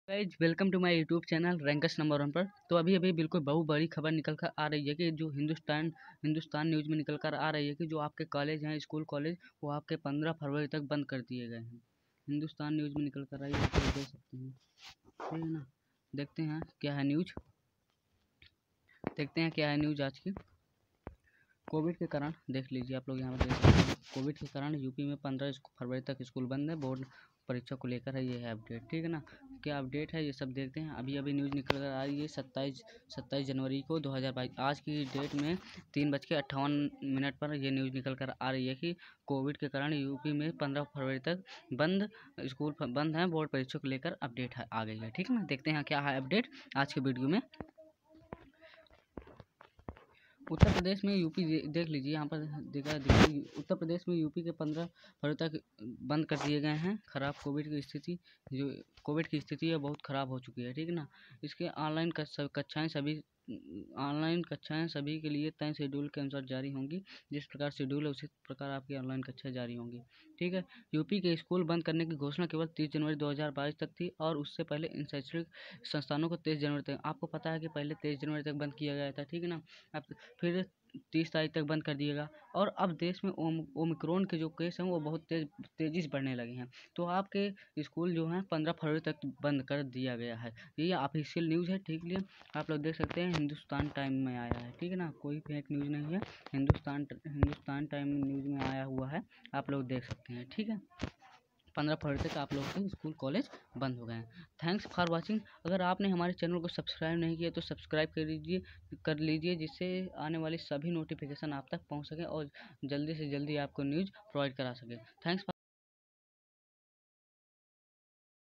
15 फरवरी तक बंद रहेंगे स्कूल और कॉलेज । जारी रहेंगे ऑनलाइन कक्षाएं
15 फरवरी तक कॉलेज और स्कूल बंद जारी रहेगी ऑनलाइन कक्षाएं

school band news today
school college band
school college band news